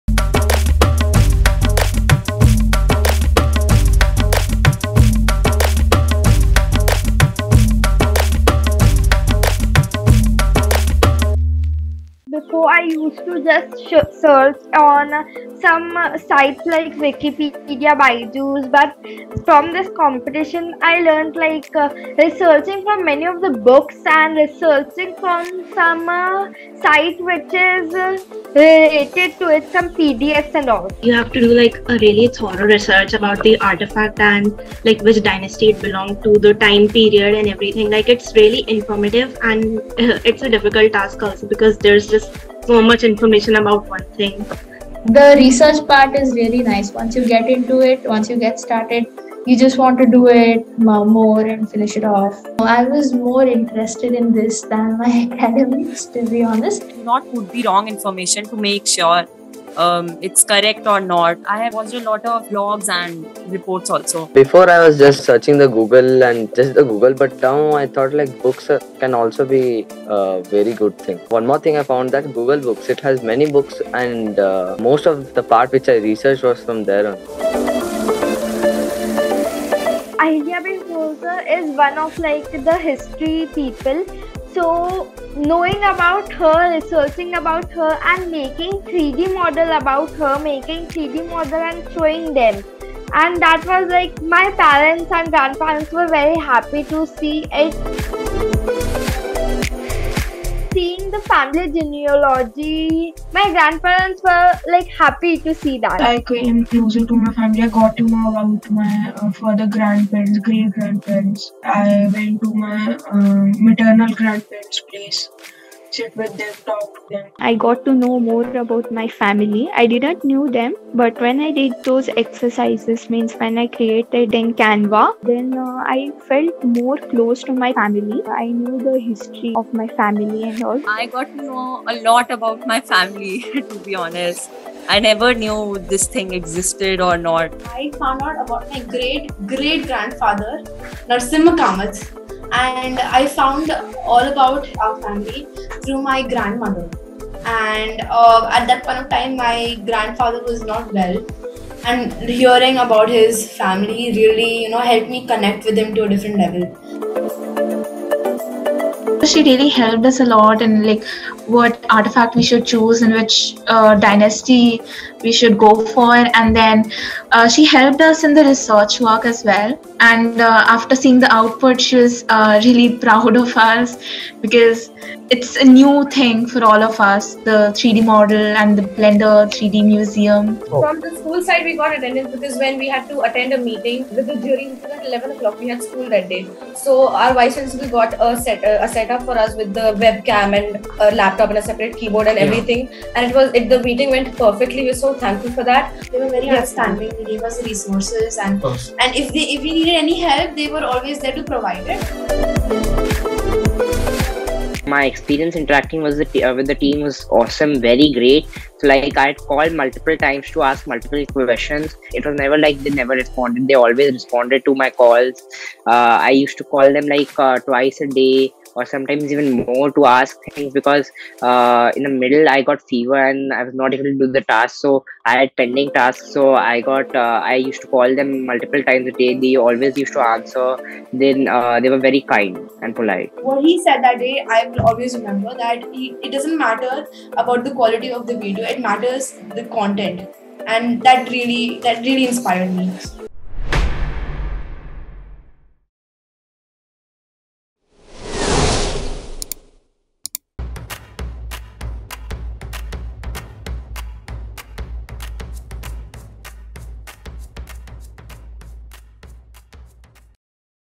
Experience of e-Yantra Virtual Museum Competition | eYVMC Edition 2
Virtual Museum Competition has exposed students to the world of research. They have done a detailed research about the artefact which they have never done before. This creates curiosity in students to know more. Parents were very happy to see the 3D artefact and the family genealogy experience brought them closer to their family. It gave them an opportunity to know about their family history and become more close to the family members. Students received a lot of support from their school teacher & a lot of guidance from e-Yantra mentors. It is a step by step competition where you have fun while you learn.

Key Points:

00:00 Developing interest in research
01:12 Google books
01:19 Parents are happy to see the 3D output
01:39 Impressions of family genealogy exercise
02:38 Support from school teacher
03:22 Interacting with e-Yantra mentors